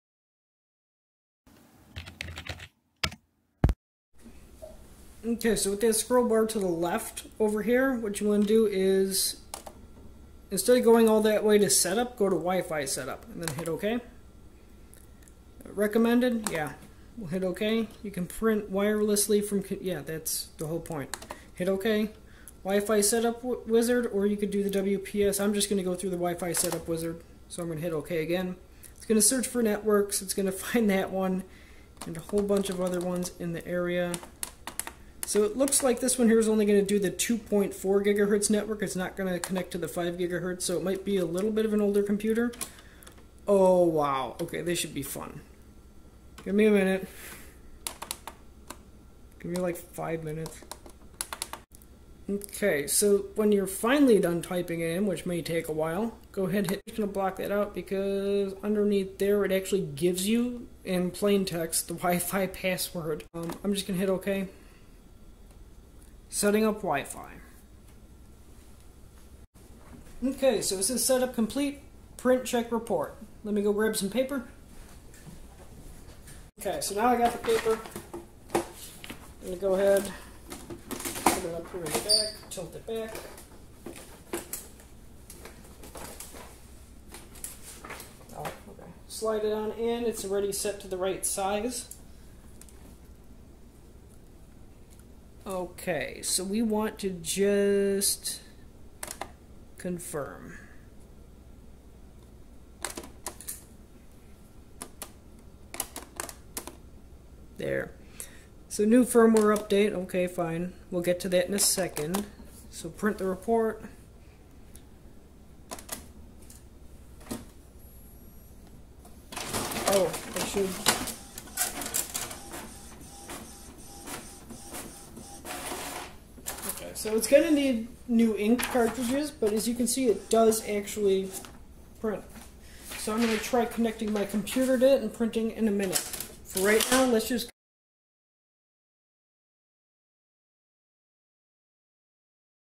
How To: Connect Epson XP-440 To Wi-Fi Network
I connect an Epson XP-440 wireless printer to my Wi-Fi Network.